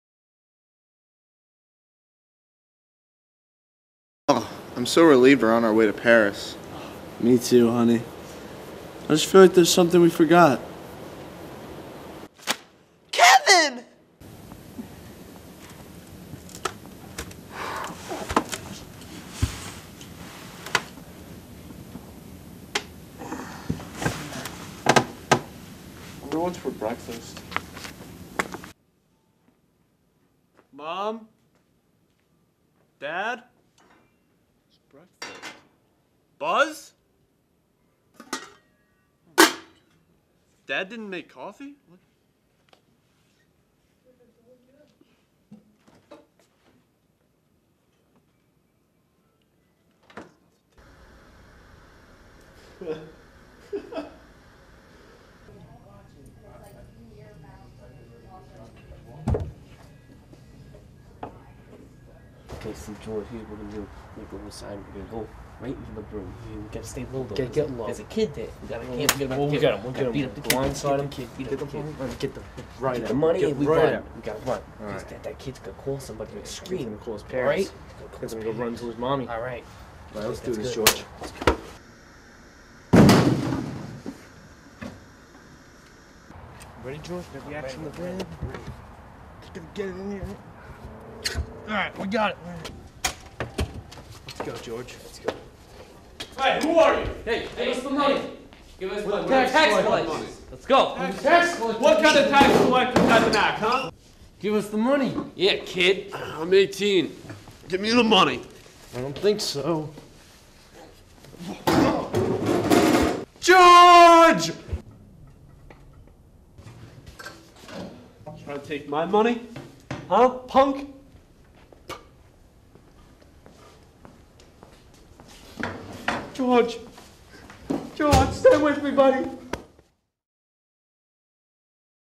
Home Alone 7
Hadden's parents leave him home alone when some robbers try and break in!